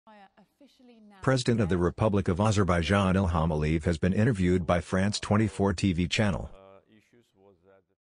President Ilham Aliyev was interviewed by France 24 TV channel
Presıdent of the Republic of Azerbaijan Ilham Aliyev has been interviewed by France 24 TV channel.